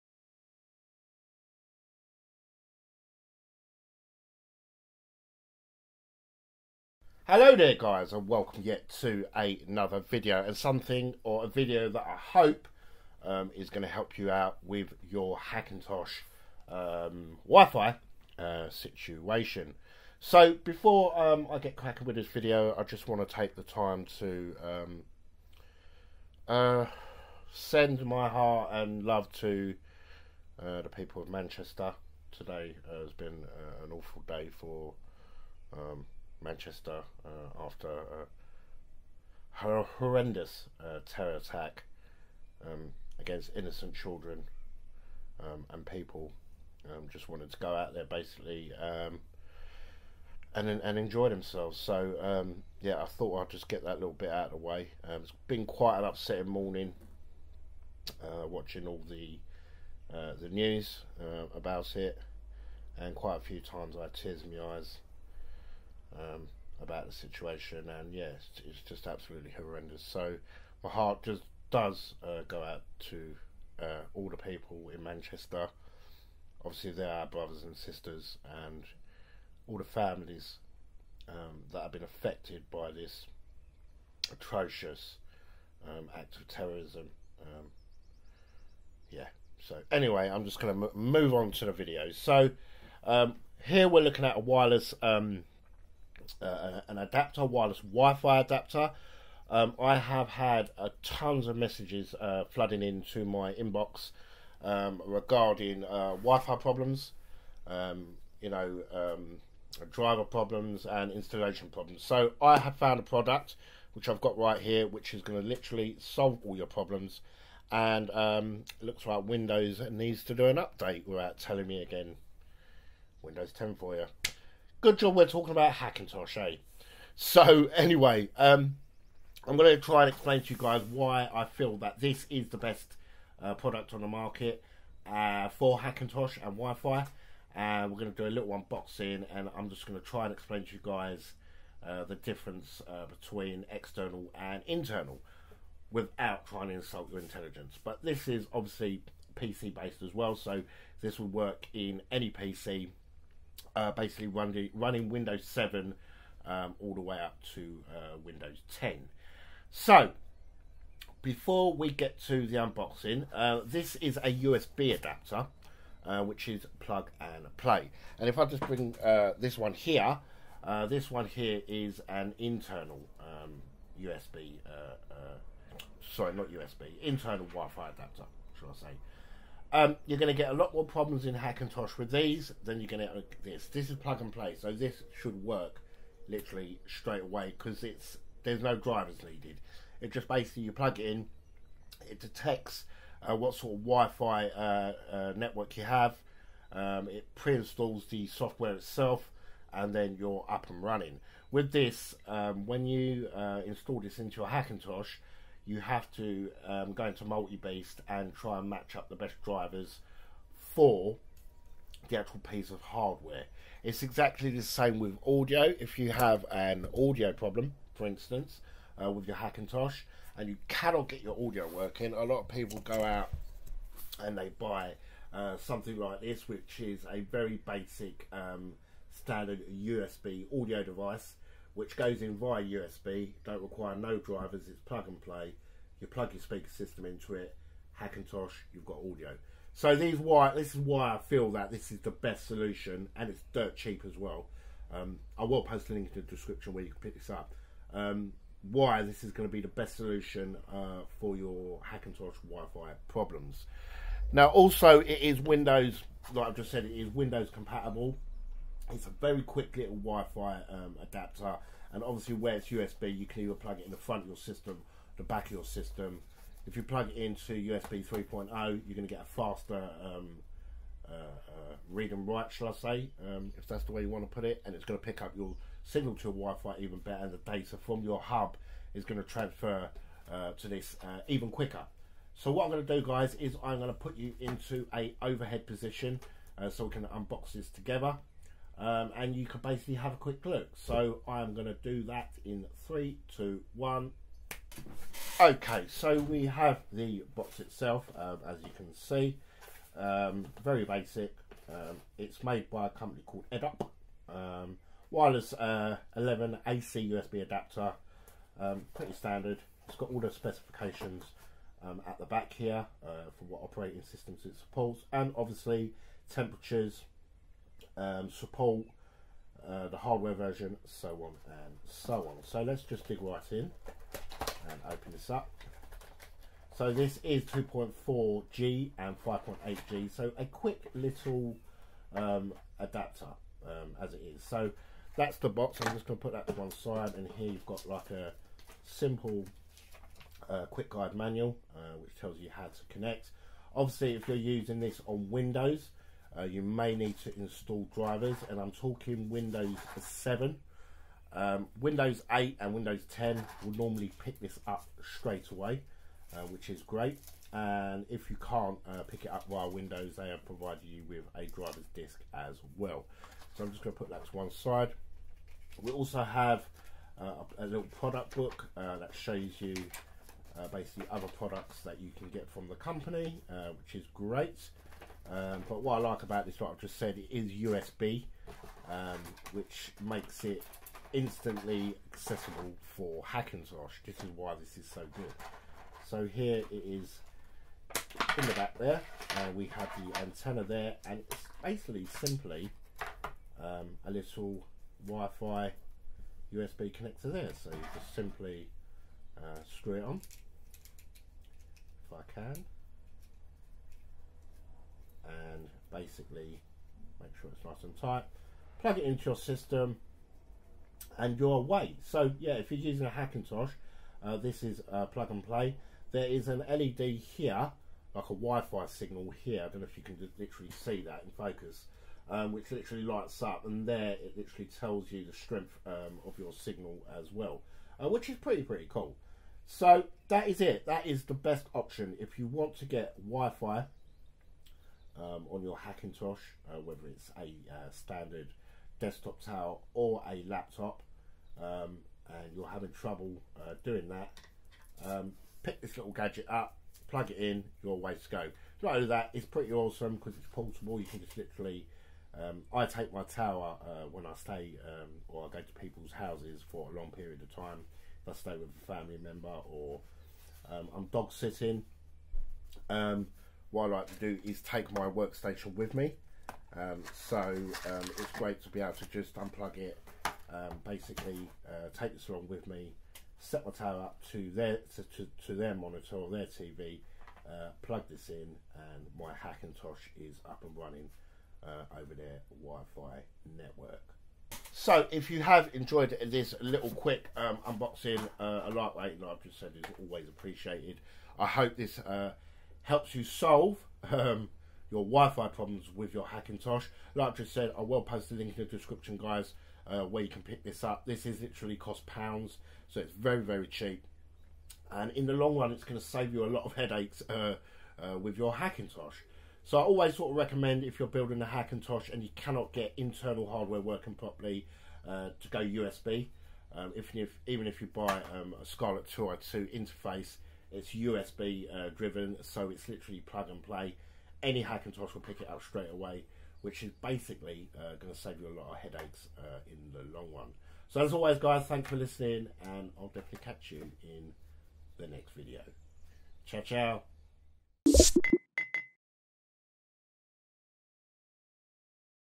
Best Hackintosh WiFi Adaptor 2017
Best Hackintosh WiFi Adapter 2017

I have found this adaptor for wifi ever hope this helps you fix your internet problems.